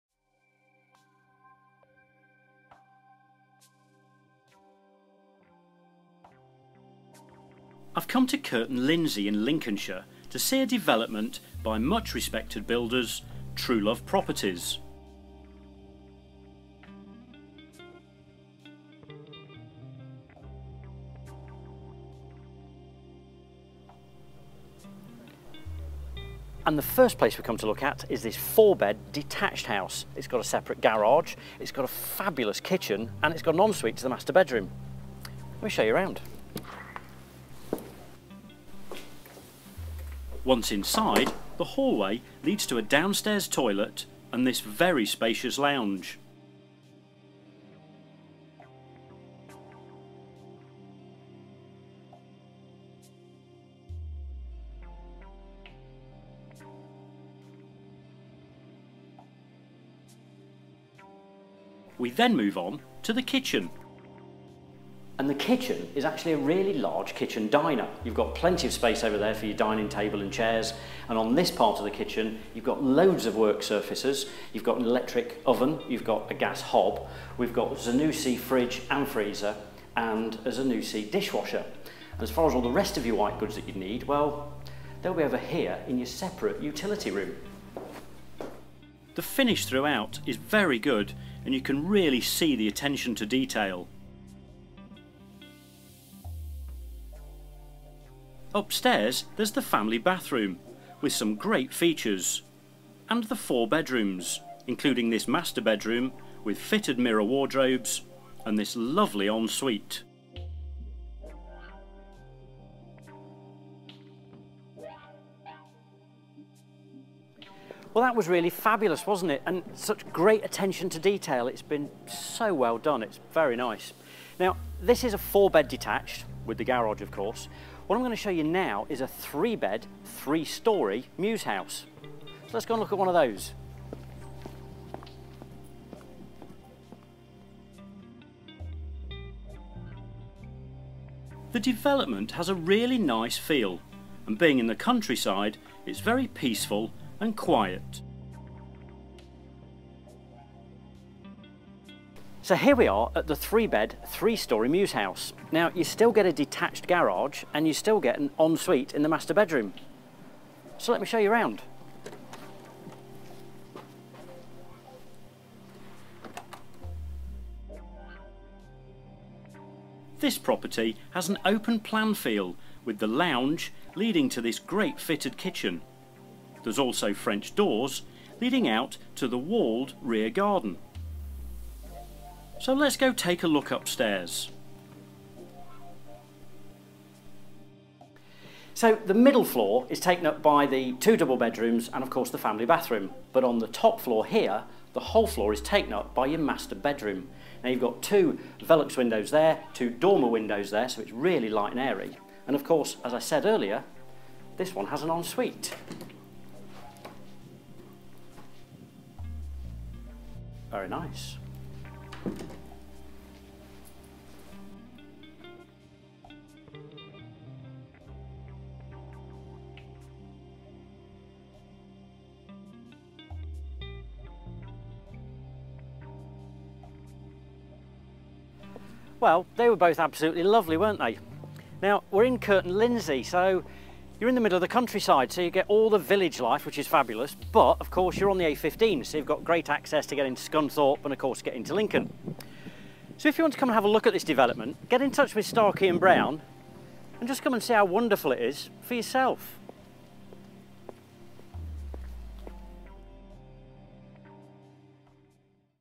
Windmill Way, Kirton Lindsey
A great development at Kirton Lindsey built by developers Truelove Properties. Quiet countryside setting yet on the A15 with great access into Scunthorpe and Lincoln.